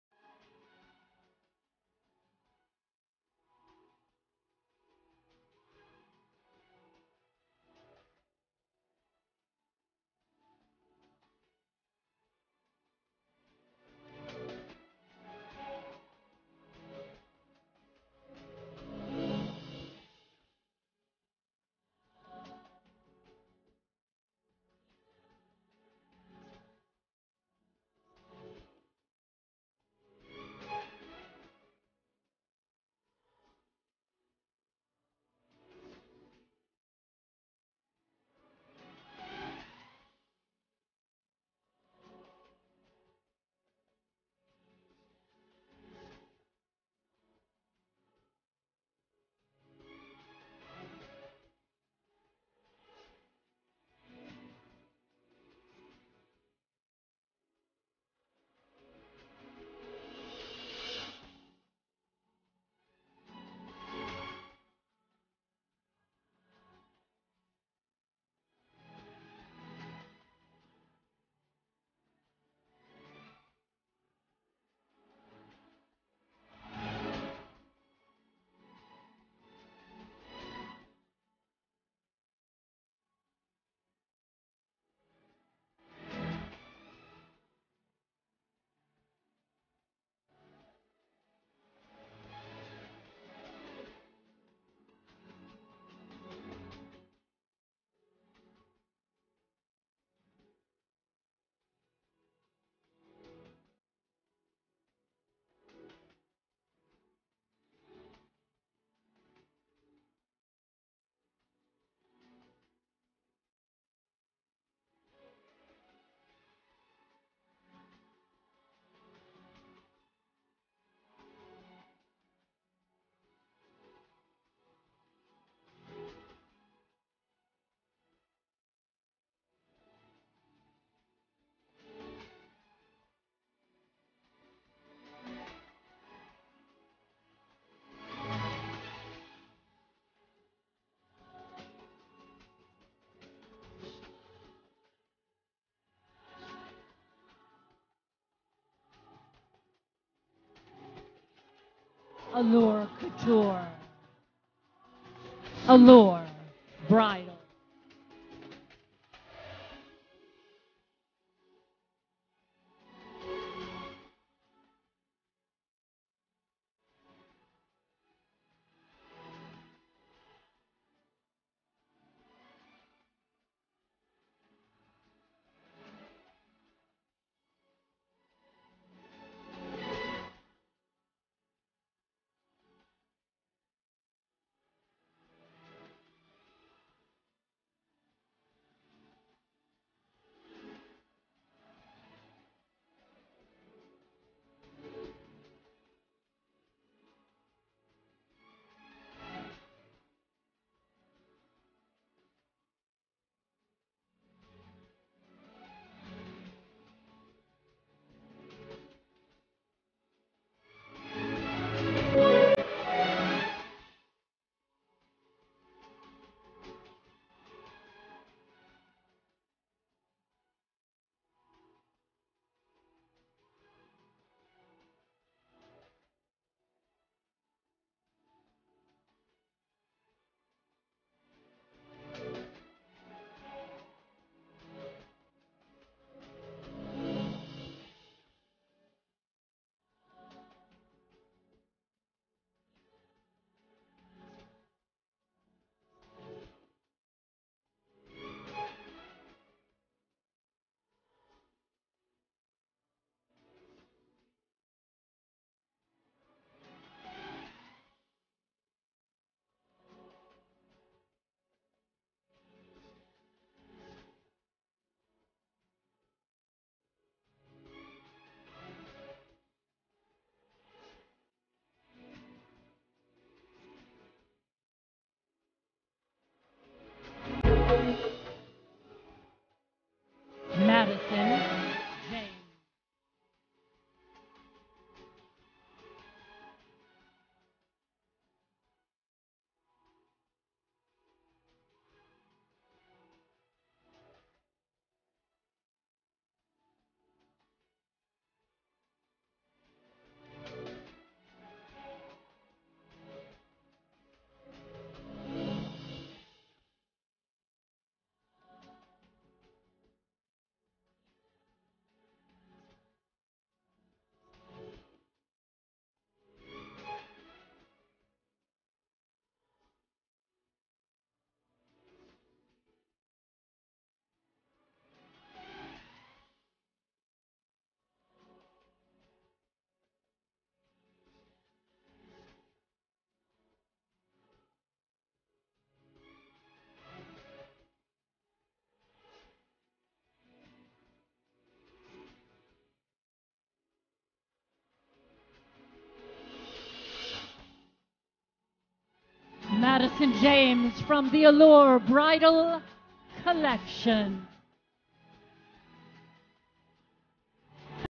Allure Bridals - Runway Show at Pier 94 during BridalMarket Fall/Winter 2015
Allure Bridals - Runway Show at Pier 94 during BridalMarket Fall/Winter 2015 from FashionStock.com : HD Fashion Runway Videos. Like this? Watch the latest episode of FashionStock.com : HD Fashion Runway Videos on Blip!

Allure Bridals - Runway Show at Pier 94 during Bridal Market Fall/Winter 2015